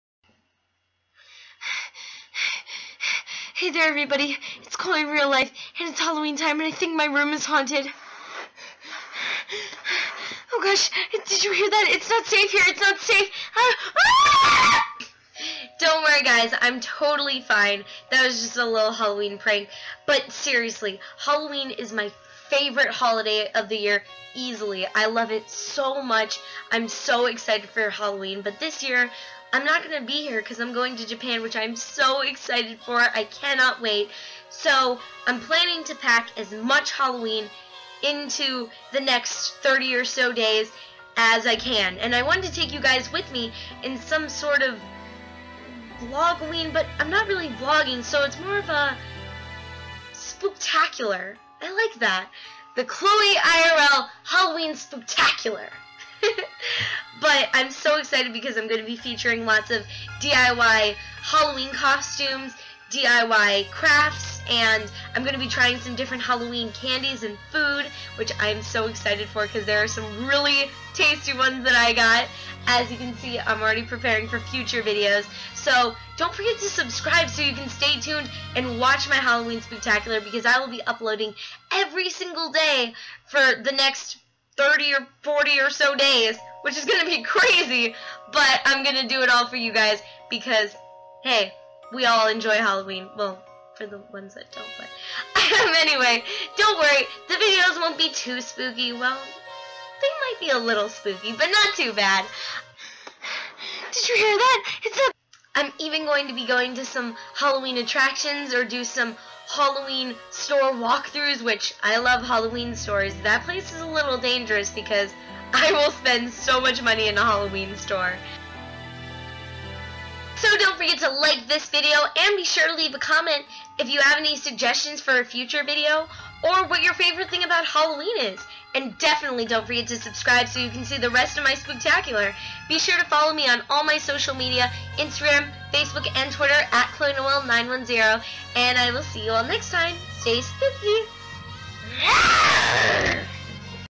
CHLOE IRL HALLOWEEN SPOOKTACULAR
Spend the next month or so with me as we count down until Halloween with cool DIYs, Spooky adventures and Yummy Treats! I love Halloween and can't wait to share this all with you! I will upload a video everyday until Halloween!